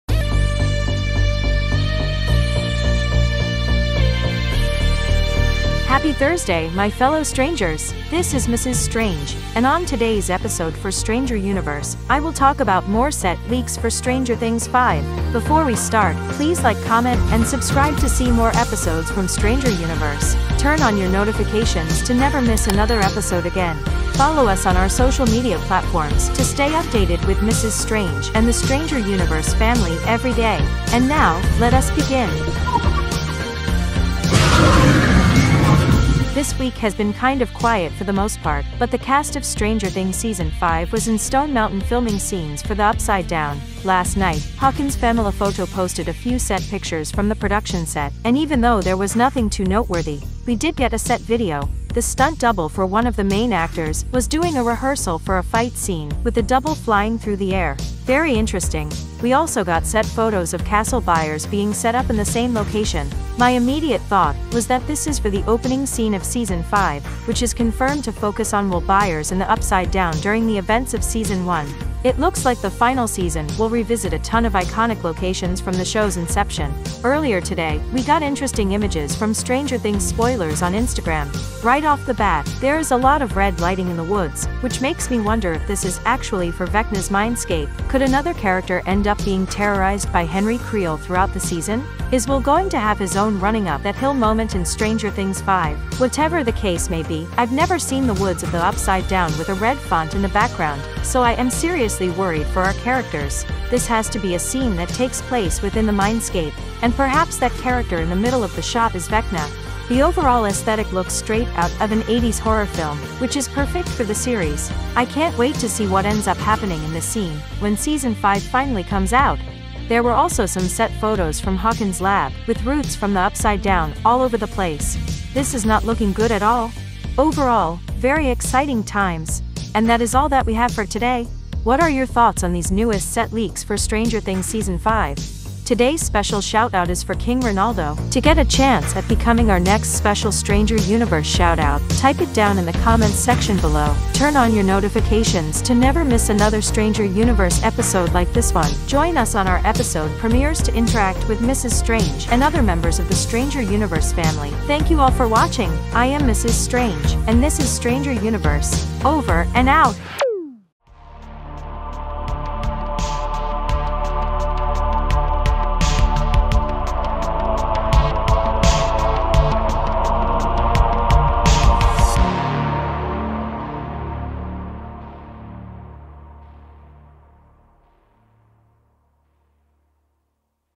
Stranger Things 5 - Vecna's Mind Lair & Castle Byers
Mrs. Strange goes over interesting set leaks for Stranger Things 5!